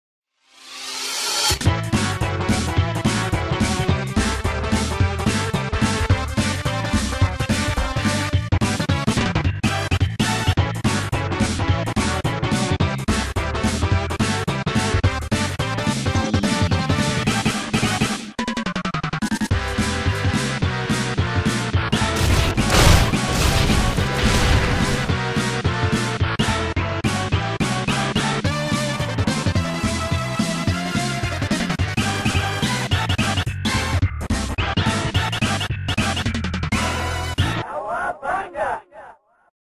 Official Turtle Thursday Intro (Ft. Zim Animation)
First of all I would love to thank Zim Animation for letting me use his 3D Intro Remake as aa part of the Turtle Thursday intro.